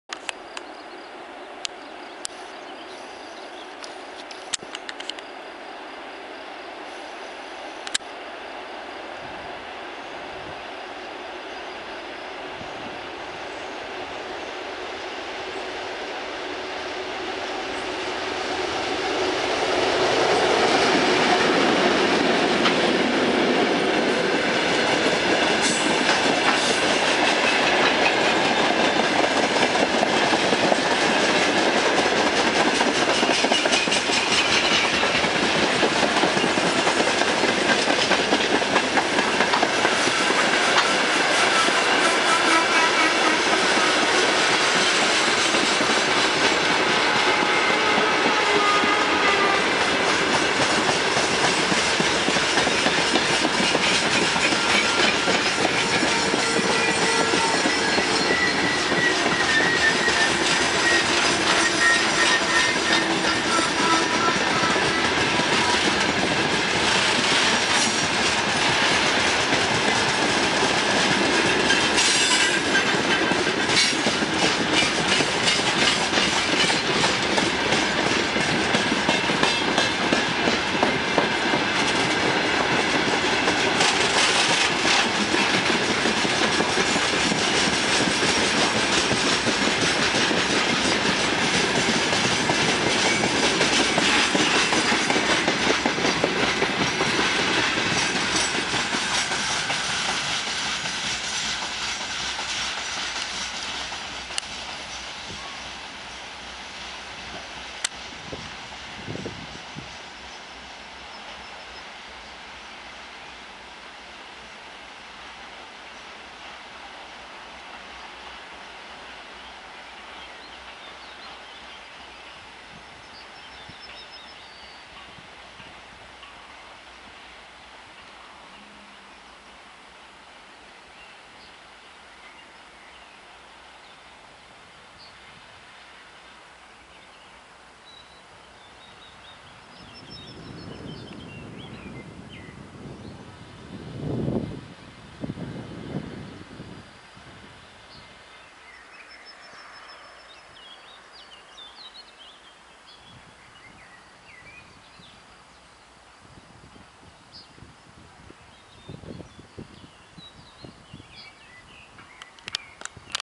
ST40s-02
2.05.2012, Rytwiany, szlak Niekrasów - Staszów LHS (Południowy). ST40s-02 wraz z ST40s-06 z ładownym wahadłem węglarek zmierzają w stronę Chmielnika.